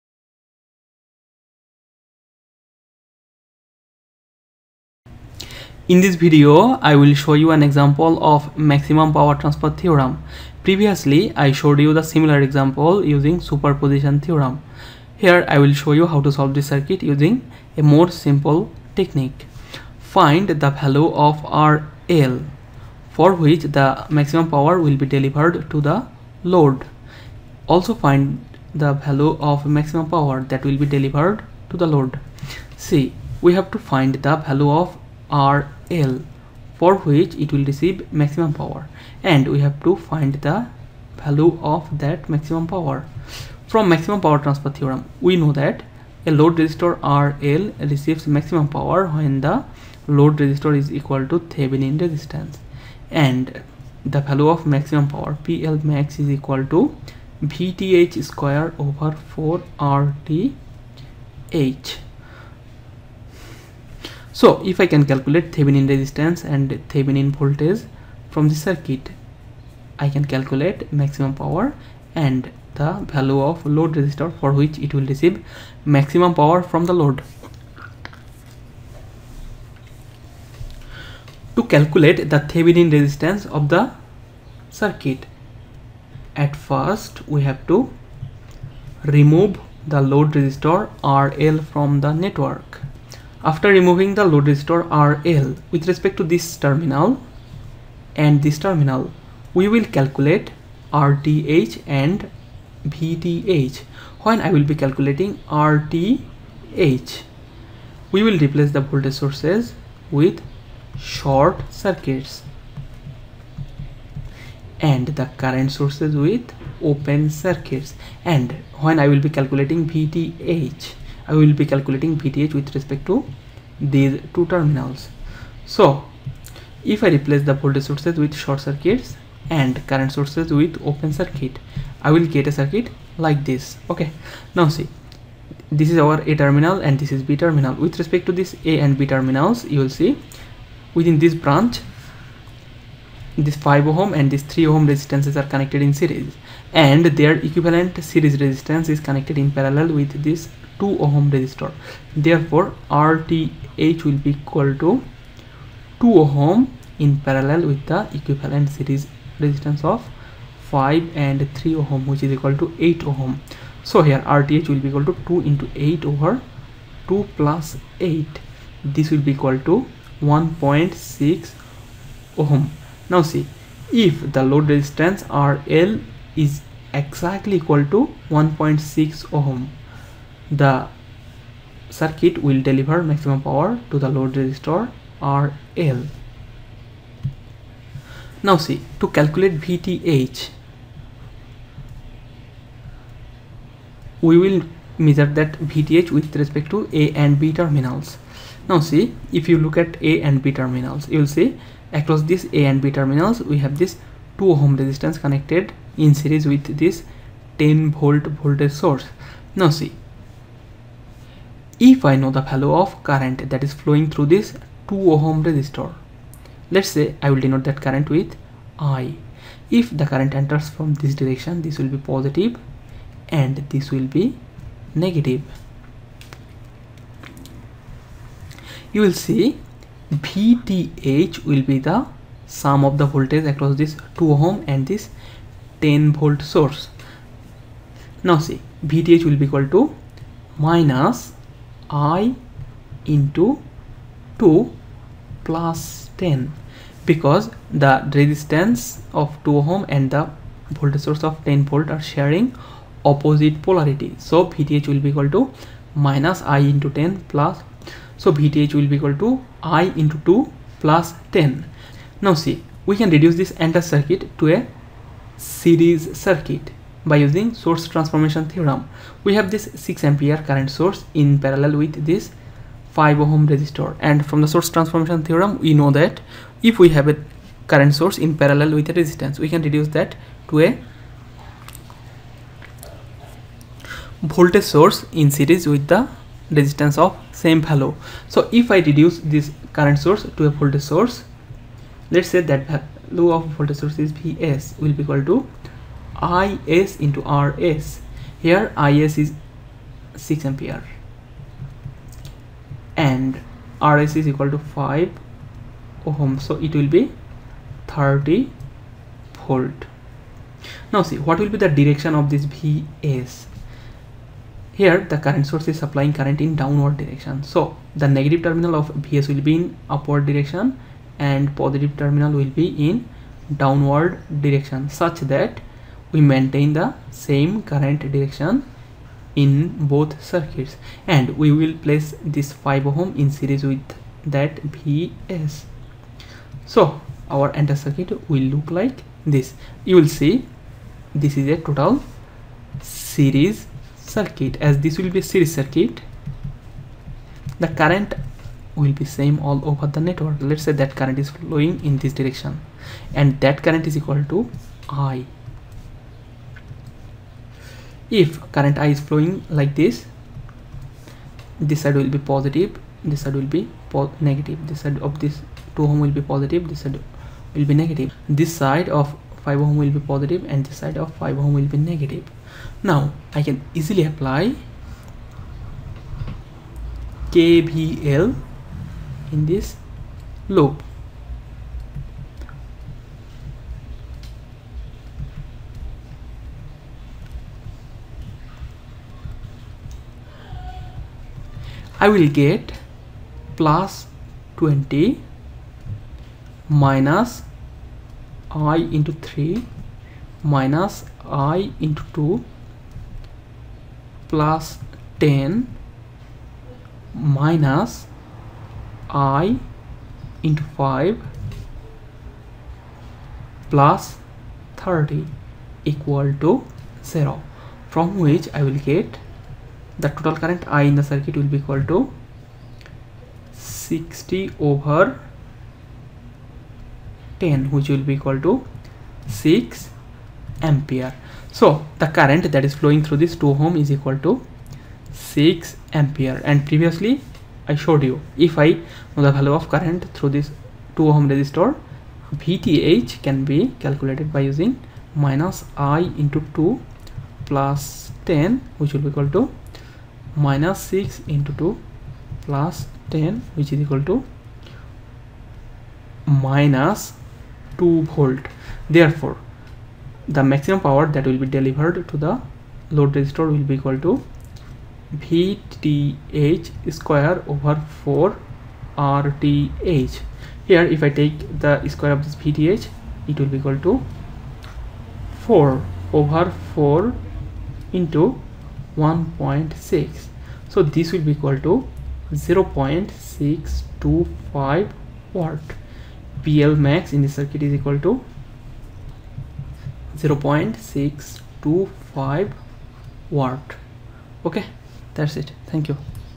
Maximum Power Transfer Theorem : Example 2 (easy solution)
A dc circuit is solved using maximum power transfer theorem.